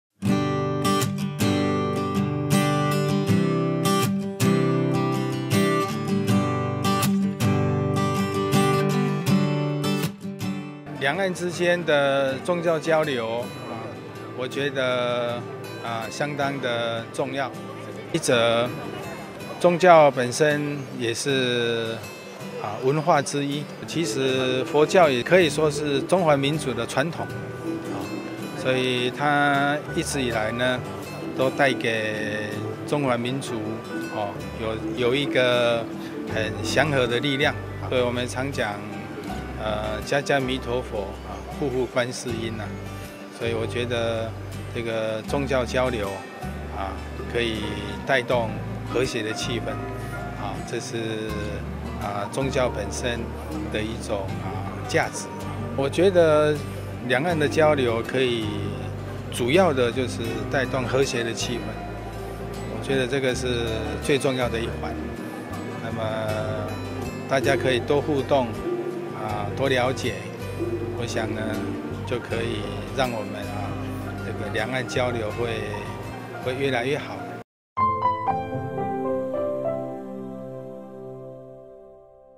心保：两岸宗教交流可带动和谐气氛 越互动两岸交流就越好
第六届世界佛教论坛正在浙江宁波雪窦山举行。10月16日，与会的台湾佛光山宗委会宗长暨佛光山住持心保在接受媒体采访时谈到，两岸宗教交流相当重要。他谈到，宗教本身也是文化之一，一直以来，佛教给中华民族以祥和力量，甚至成为人心中的依靠，“家家弥陀佛，户户观世音”，两岸宗教交流可以带动和谐气氛，越互动，两岸交流就越好。